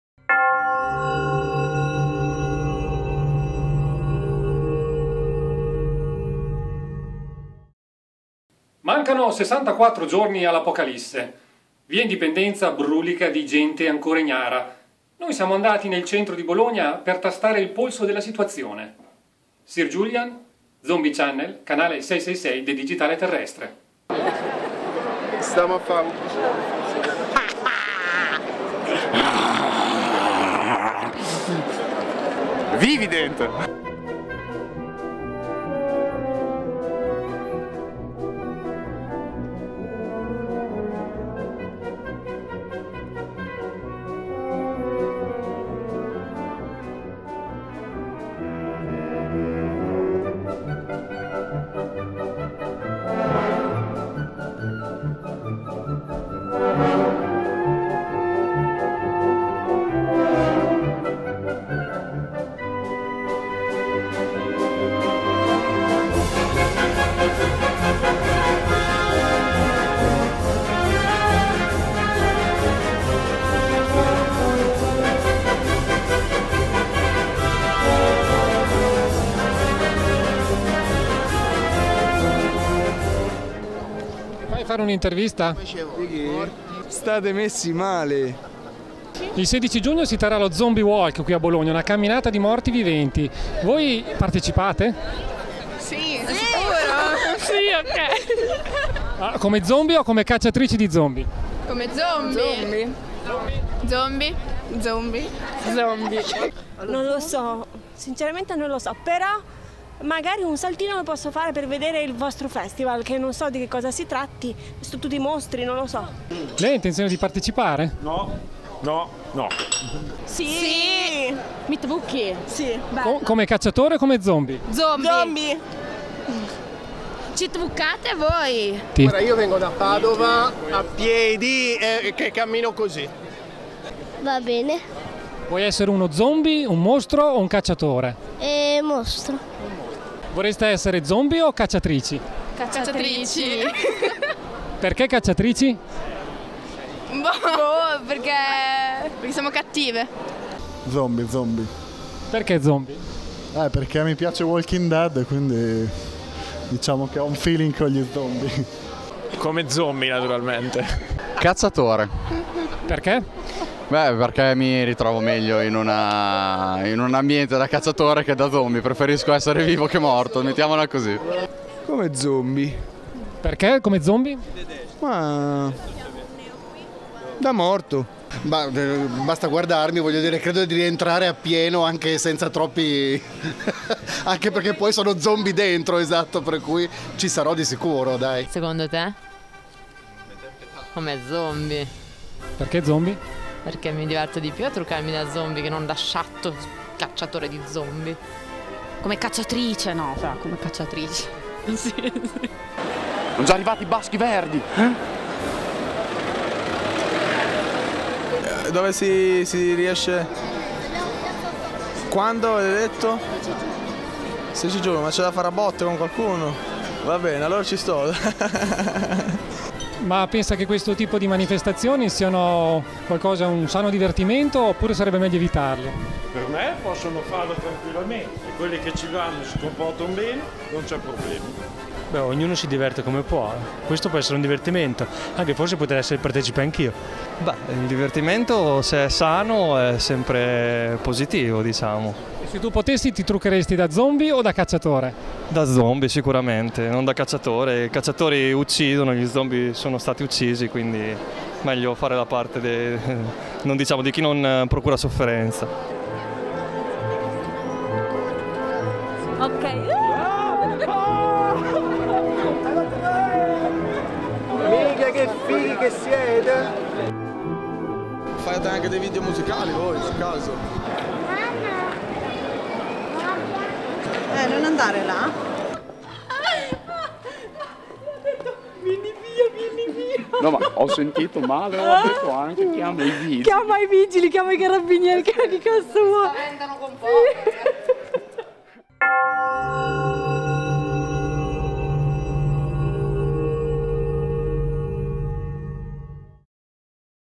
Sarai zombie o cacciatore ZOMBIE CHANNEL LIVE
2 mesi prima dell evento "Zombie Walk" a Bologna Giuliano Lugli intervista ignari passanti su "ZOMBIE CHANNEL"